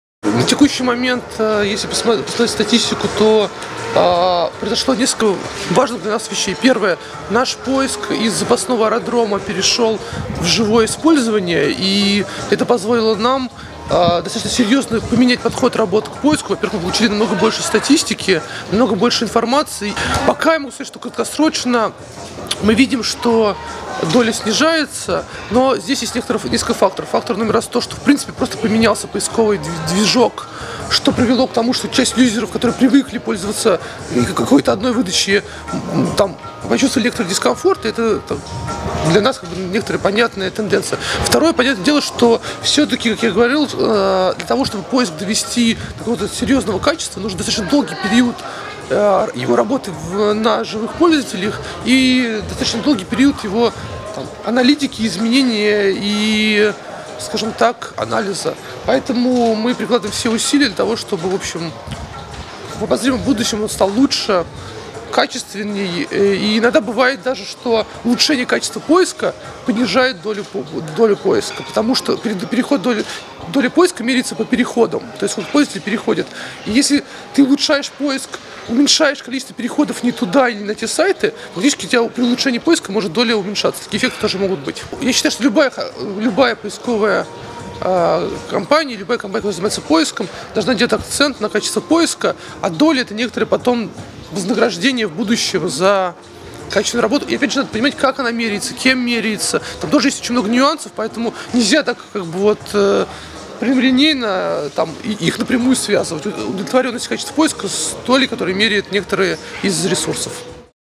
Дмитрий Гришин: если вы «Аватар», то можете брать 99% рекламной выручки
Mail.ru готов делиться доходами от рекламы с контентными компаниями, рассказал в интервью Infox.ru Дмитрий Гришин, генеральный директор интернет-холдинга. За 2009 год компания заработала больше $126 млн.
Дата публикации: 25 июня 2010 12:30